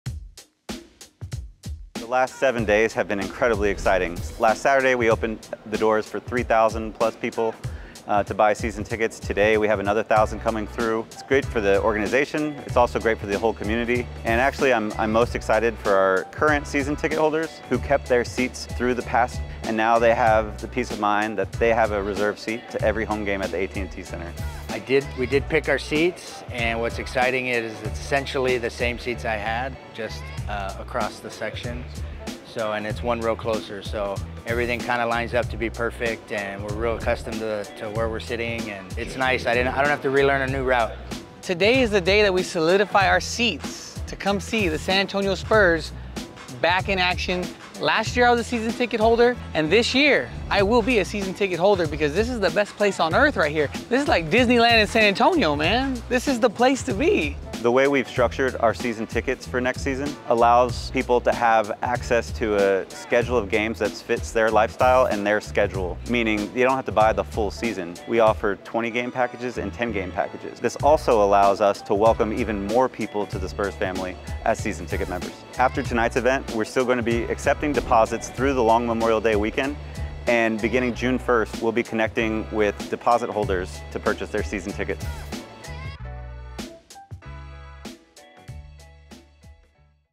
San Antonio Spurs Season Ticket Members Select-A-Seat Event
It's the place to be!

Our Spurs Season Ticket Members had the opportunity to secure their seats for the upcoming '23-24 season at our Select-A-Seat Event!